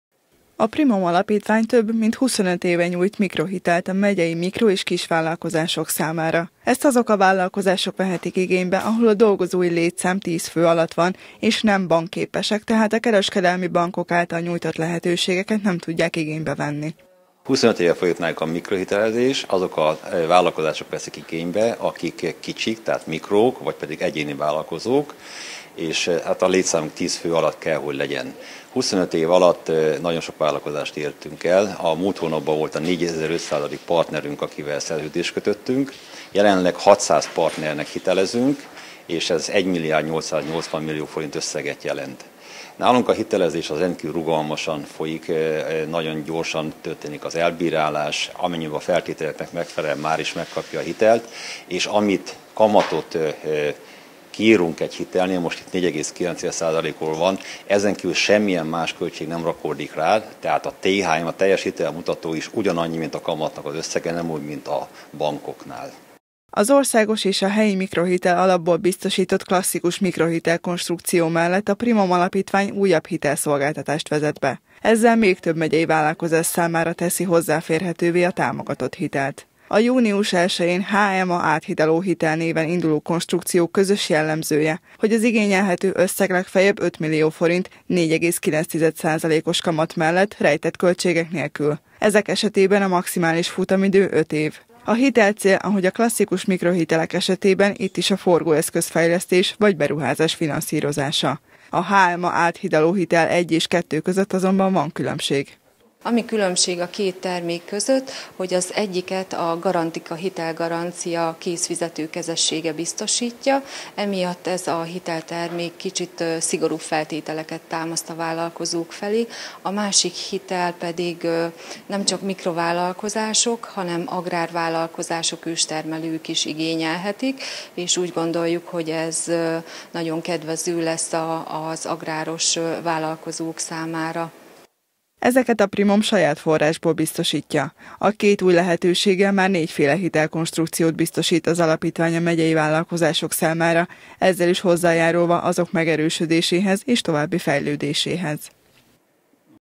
Új hitelkonstrukciók - Kölcsey Tv
Június elsejétől új hitelkonstrukciókat kínál a PRIMOM Alapítvány. A “HMA Áthidaló Hitel” 1 és 2 jellemzője, hogy legfeljebb 5 millió forint igényelhető 4,9%-os fix kamattal, 5 éves futamidővel. A különbség a kettő között, hogy a 2-esnél a Garantiqa Hitelgarancia Zrt. készfizető kezességet bíztosít, így annak szigorúbbak az igénylési feltételei.